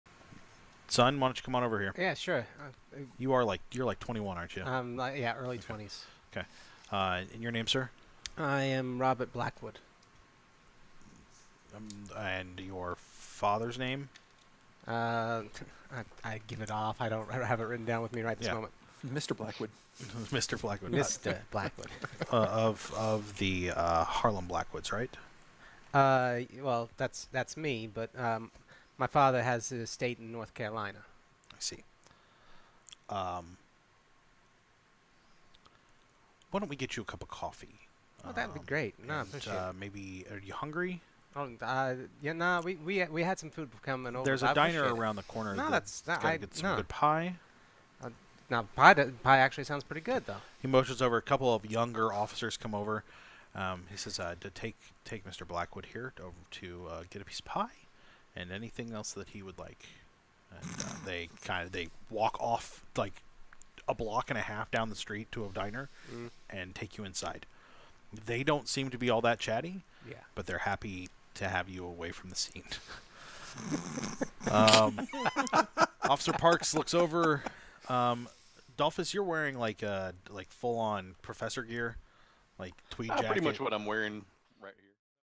Call of Cthulhu - The Legend of Pie
There is a meme on the server that only the old timers know; and now so do you! Long ago, before Rook and Rasp became the channel it is now, Illuminated Page ran on Quiet Gentlemen and we had the dream of being a mostly Call of Cthulhu focused streaming platform (oh, how times change).

Then "Pie" came up from our GM to Bobby Blackwood - and since then Pie has appeared as a running gag referring to this line from the original episode 1 of Illuminated Page!

This series is a little bit of a "rough start" for the rest of what Rook and Rasp is now - so pardon the sound and video quality!  Also this episode is PRE-Covid before JKR came out against the Trans community, so pardon the decor, please!